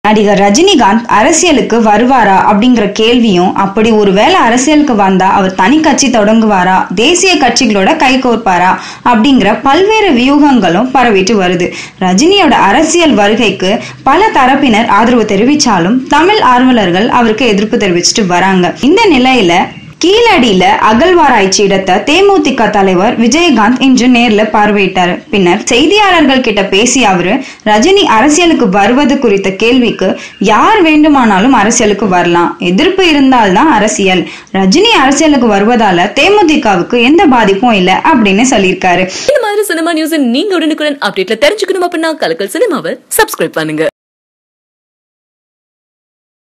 Will Rajini's political entry affect DMDMK's fortune: Vijayakanth clarifies
ரஜினி அரசியலுக்கு வந்தால் தேமுதிகவுக்கு பாதிப்பா? விஜயகாந்த் பதில்!